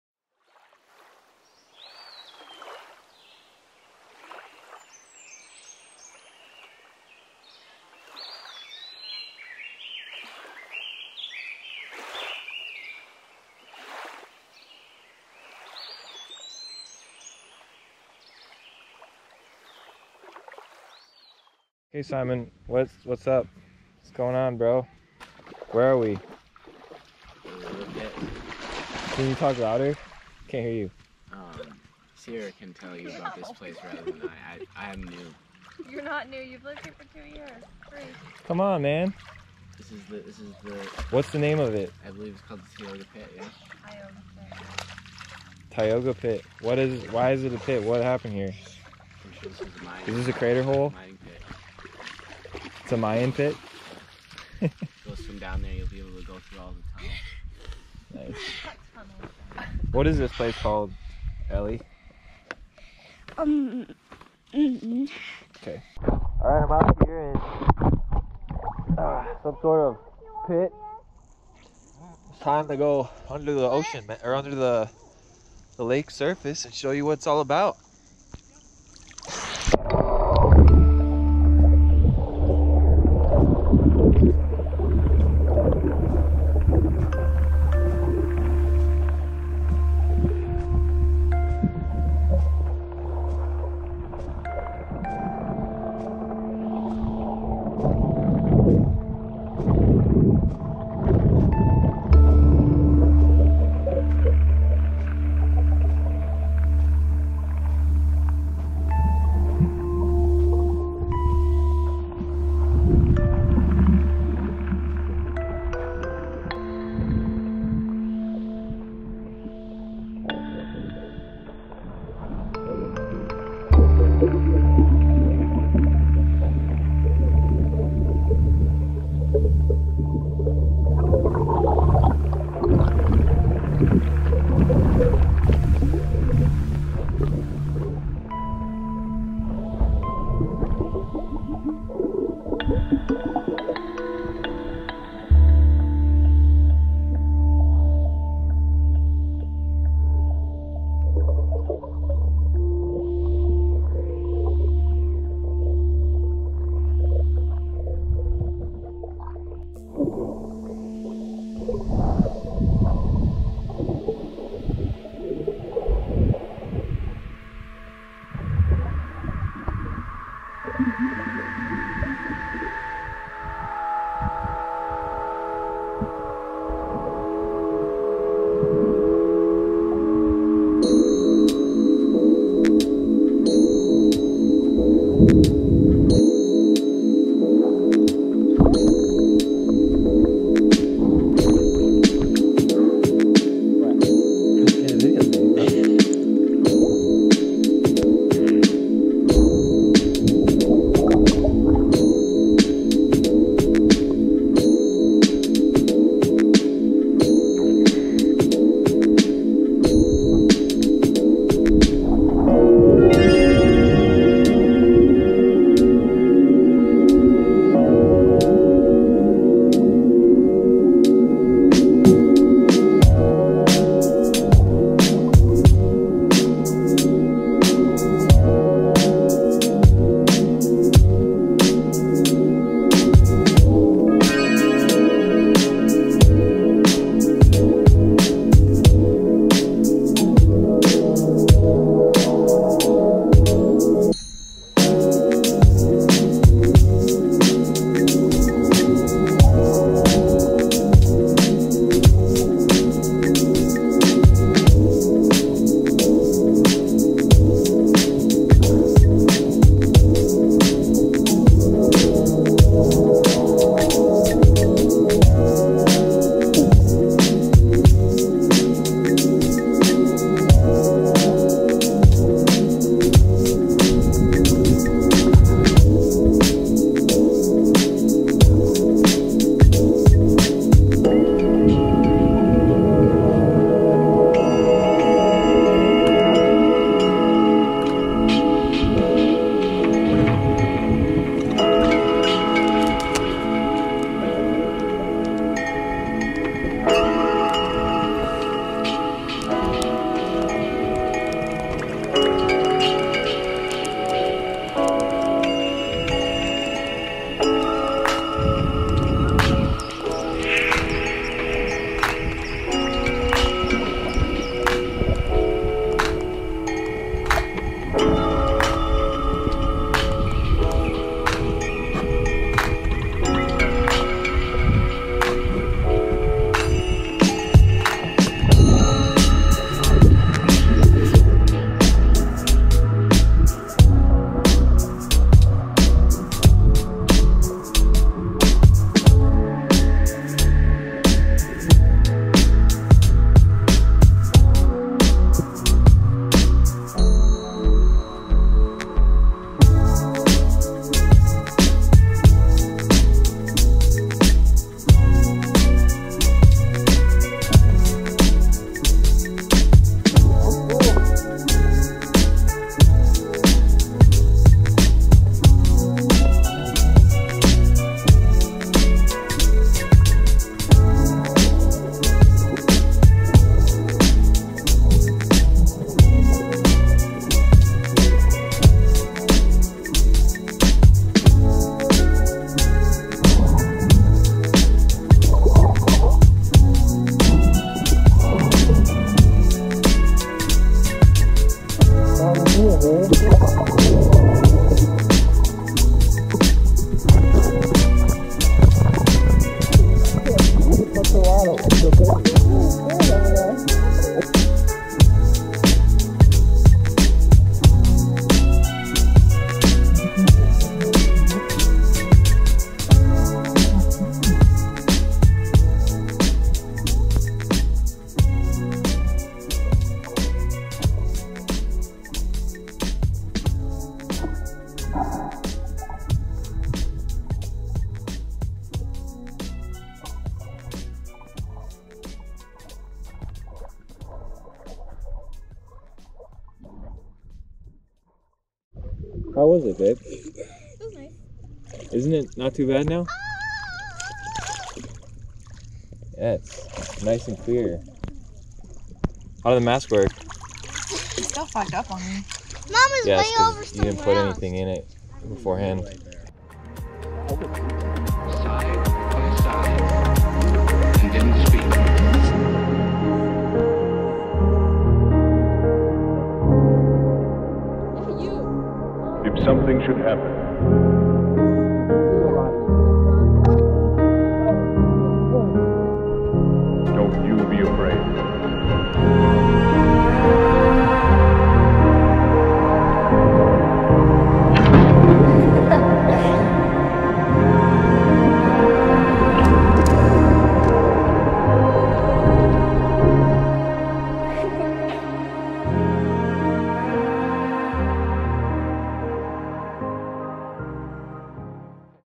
Snorkeling and Exploring Taioga Pit Recreation Area in Minnesota
This past summer, Nicole and I visited my brother in Grand Rapids Minnesota. Simon took us to Taioga Pit Recreation Area which used to be an iron ore mine back in the 1950s. It is now a beautiful recreation site and offers some crystal clear water to swim in. Although the water is cold (70 degrees) I brought my snorkeling equipment to explore the clear aqua colored water. I even convinced Simon to join me. Hope you enjoy!